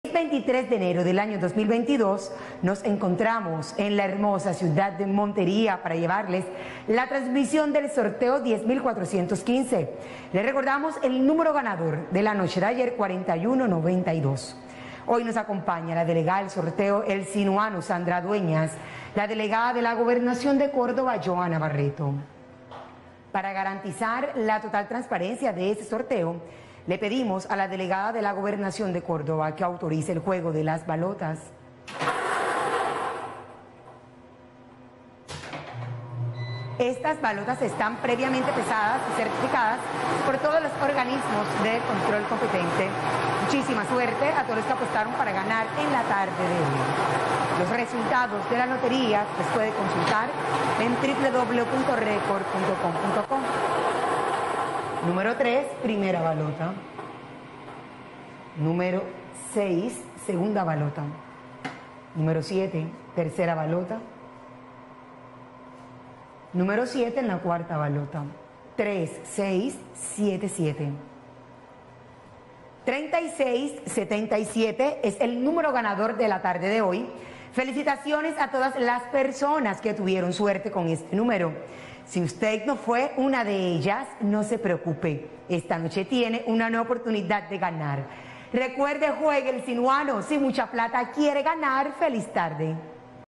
SINUANO DÍA Resultado del Domingo 23 de enero de 2022 ✅🥇🔥💰
Hola buenas tardes, te presentamos los resultados de las loterías y chances en Colombia, vamos con el Resultados del Chance Sinuano día del Domingo 23 de enero de 2022.